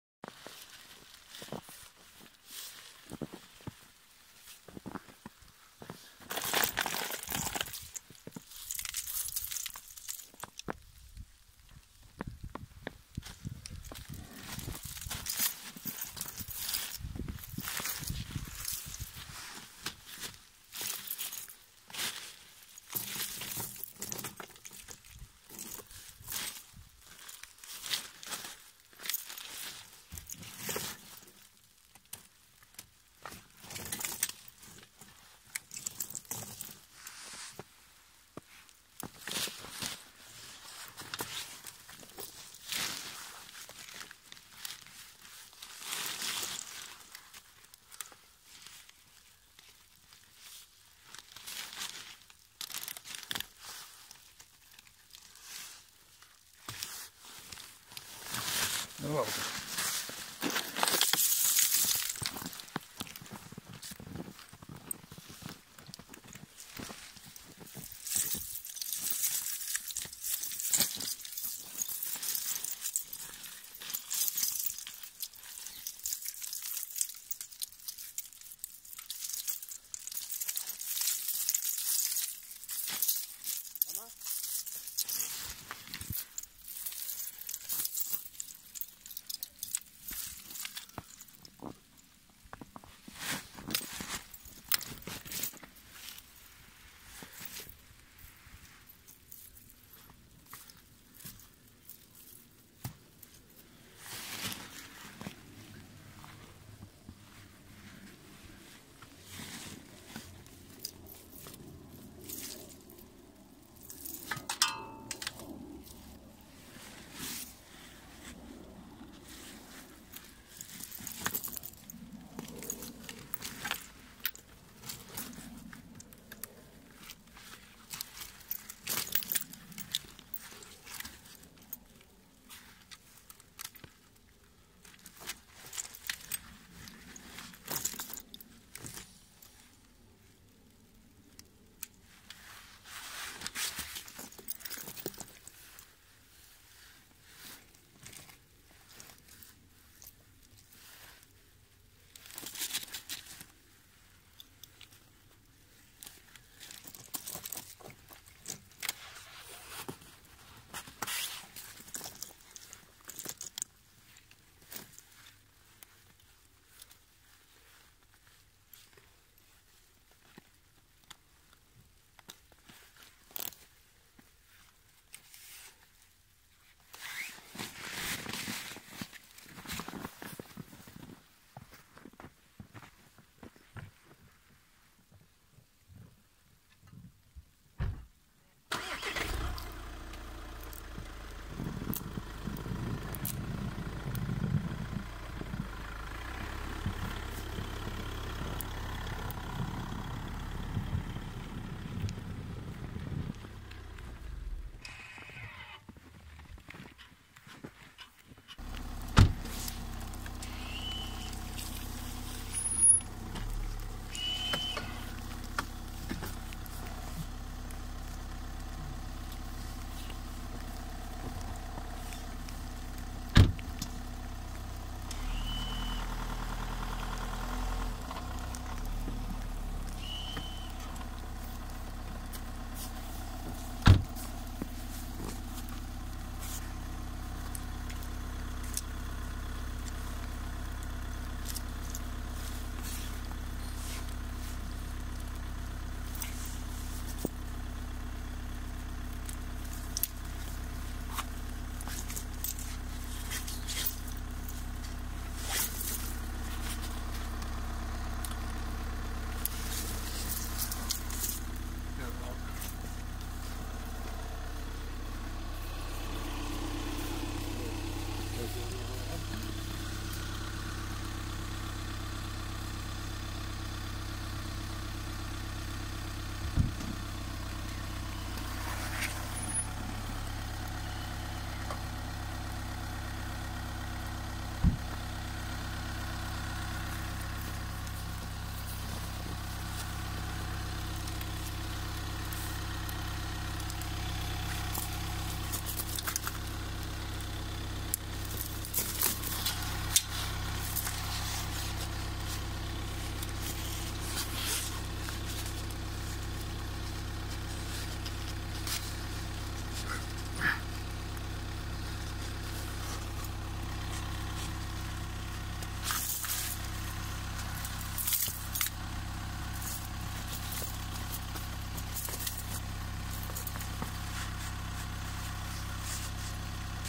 How to fit & remove snow tire chains Thule König XG-12 Pro
Installation and removal of the Thule chains in snowy conditions.
Few tutorials describe how to remove them.
To buy in Amazon USA
Para comprar en Amazon España
Thule König XG-12 Pro (240) snow /tire chains
235/50 R18 tires